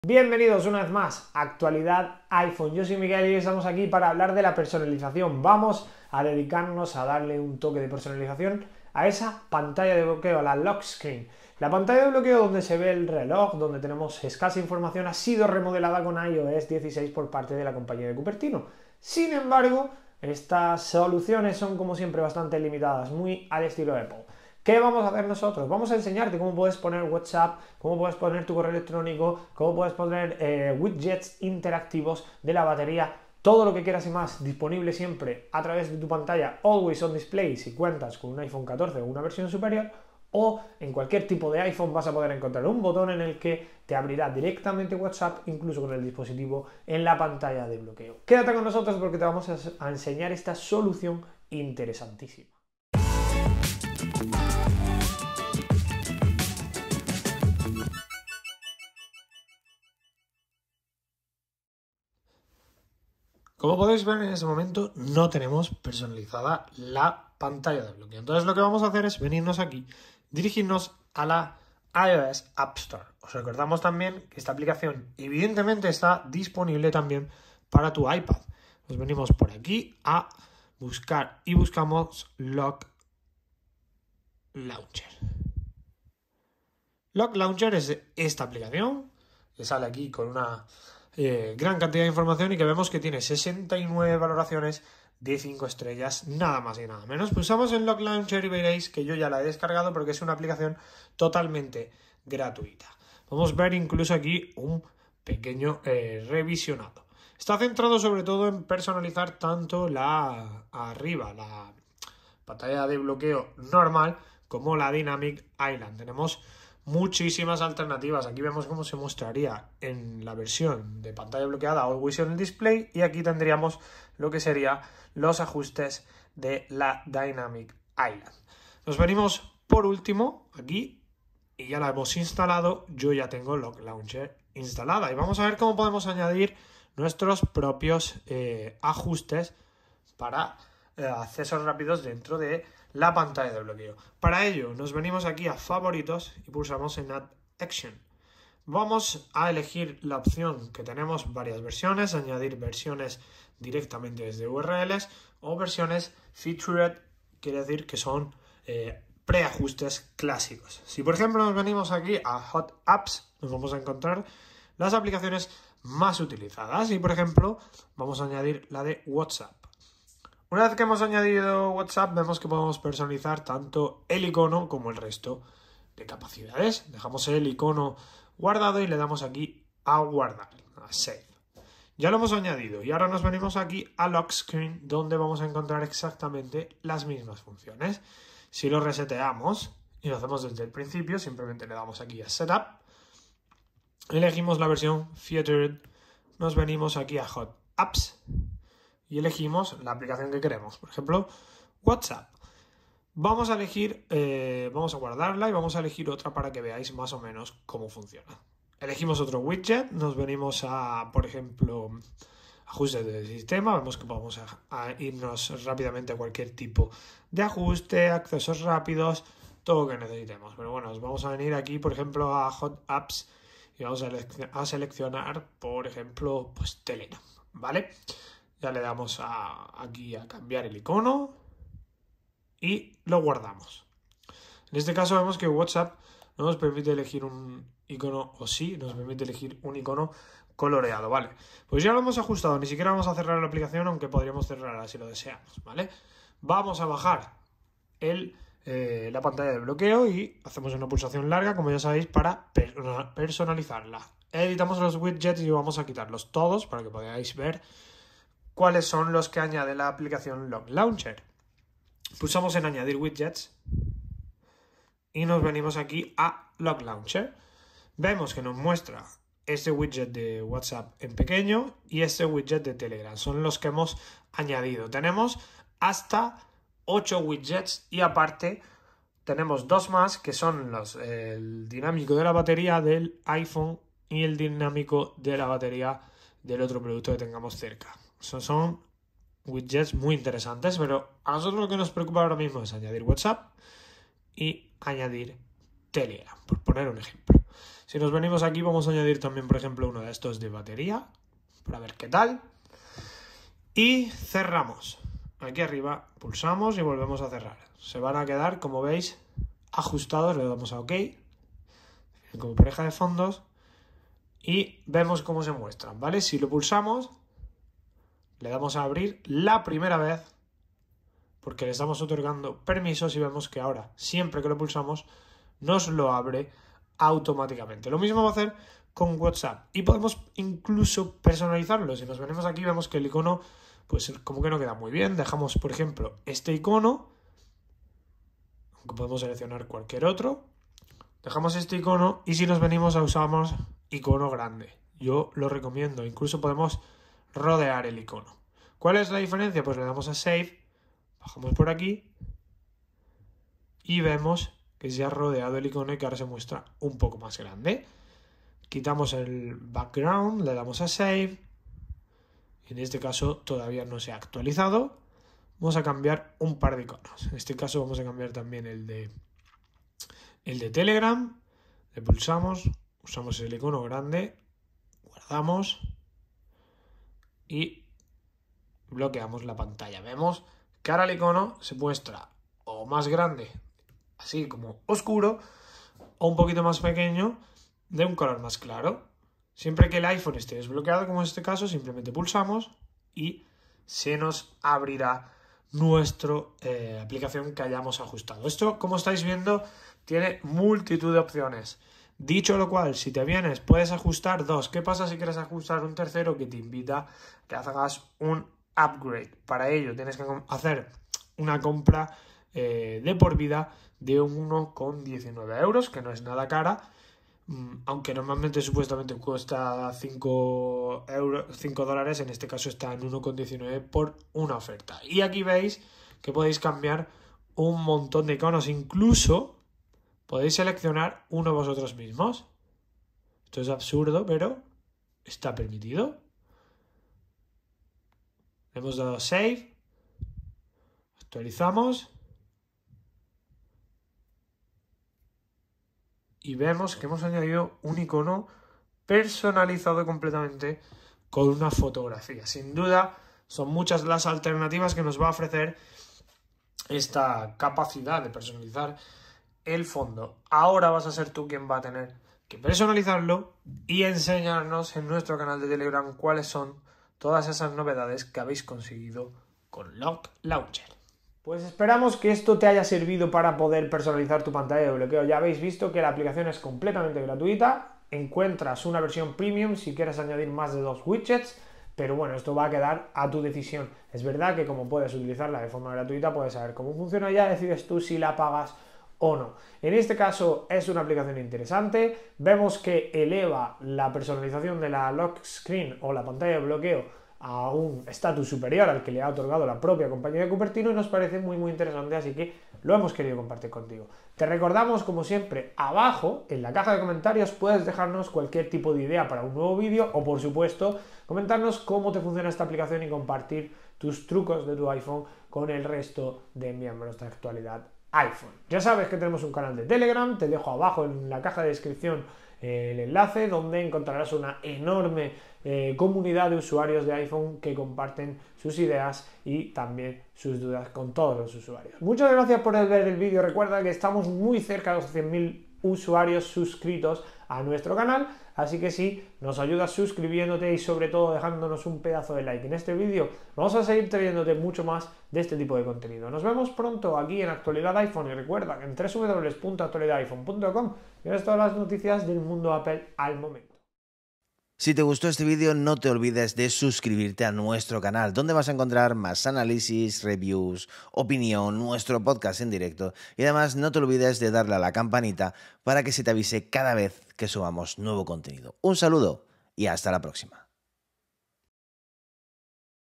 Así puedes tener widgets de WhatsApp en la Pantalla de Bloqueo
Los ajustes de personalización de la pantalla de bloqueo de iOS 16 son claramente mejorables, por lo que gracias a Lock Launcher, una aplicación totalmente gratuita, vas a poder tener tener un Widget de WhatsApp, Instagram o cualquier otra aplicación.